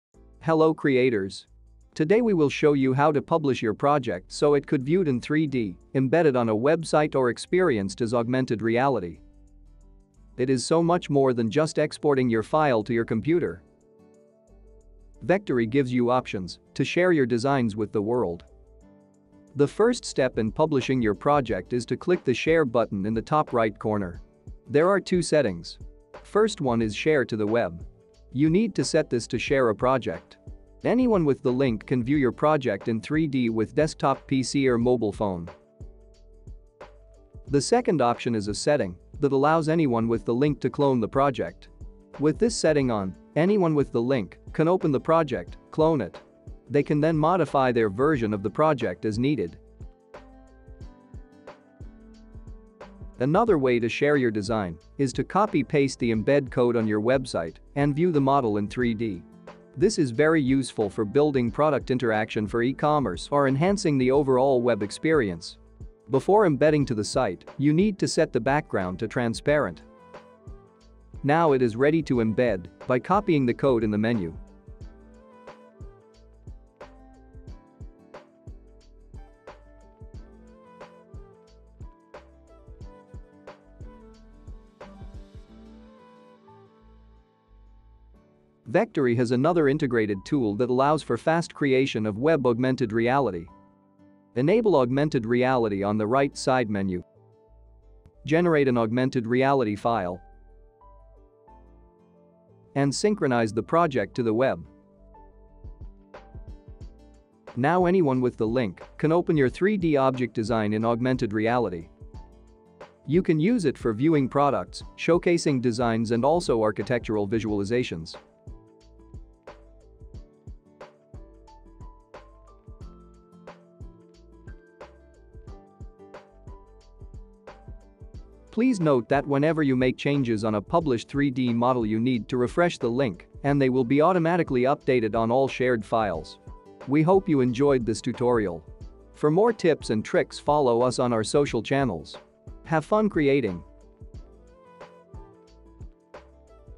Sharing 3D projects on the web | Vectary tutorial
Get your designs ready and share them with the world, embed them on your site, or create an AR. This tutorial explains the different steps to get you there in Vectary.

0:00 Introduction
0:22 Share 3D as a link
0:35 Clone settings
0:52 Embed on the web
1:06 Set a transparent background
1:28 Augmented Reality
2:10 Synchronize changes